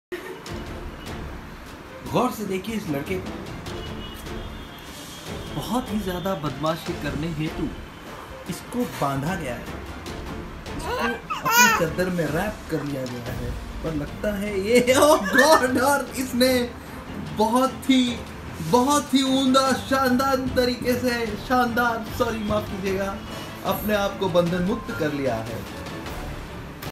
Naughty Shaurya Unwrapping himself from a towel trap.
Funny video of naughty baby Shaurya 👶